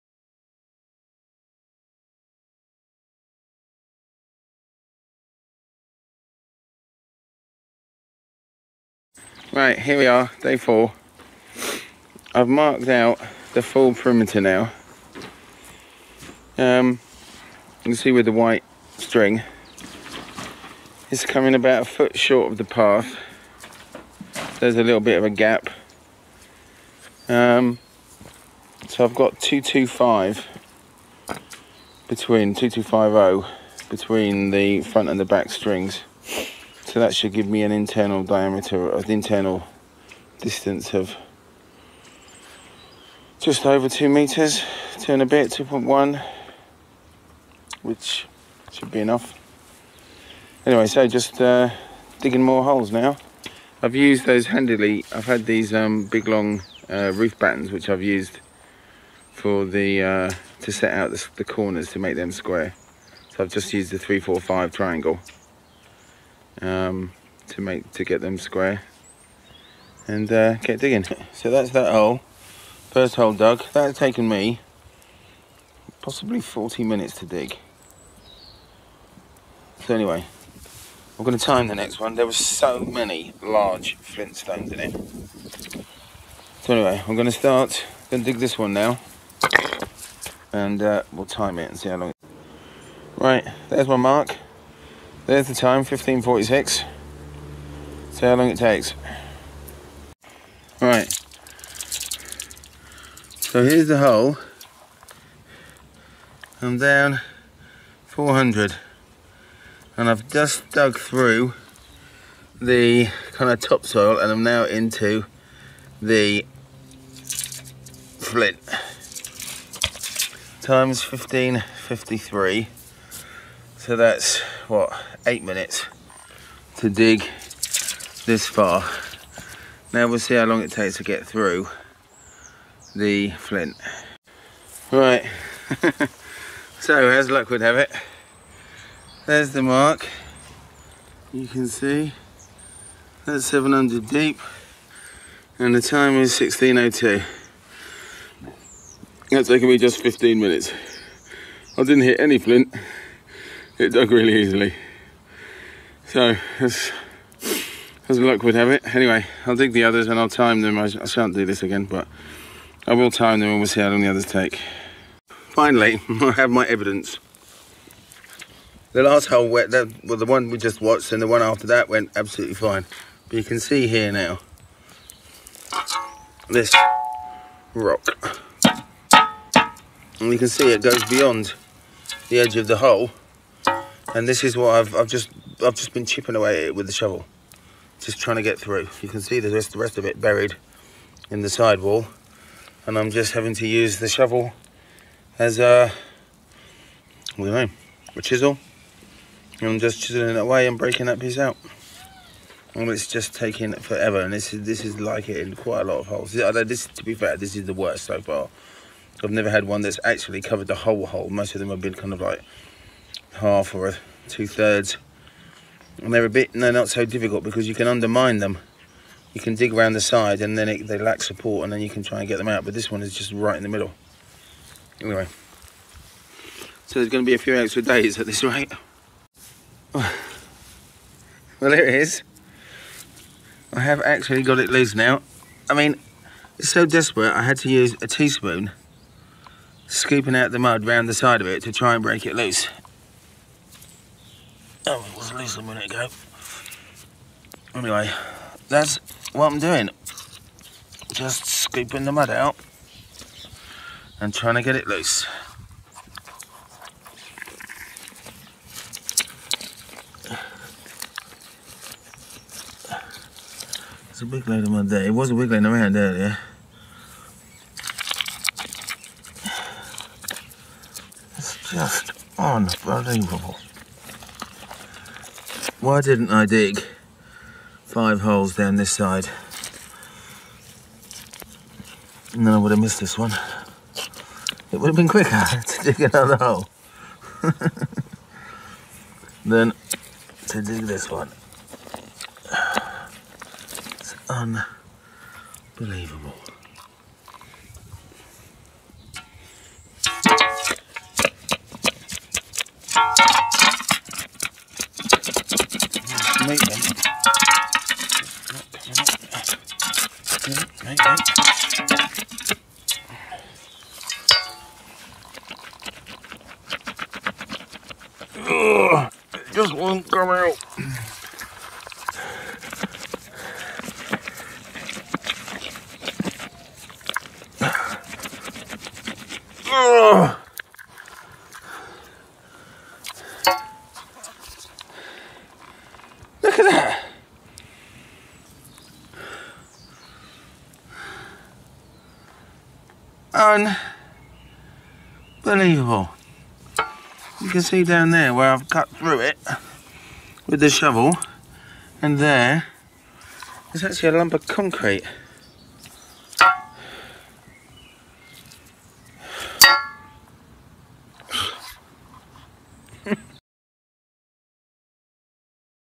GardenRoom Day 4
I am building a 6m x 2.5m garden room with underfloor heating powered by roof mounted solar thermal panels.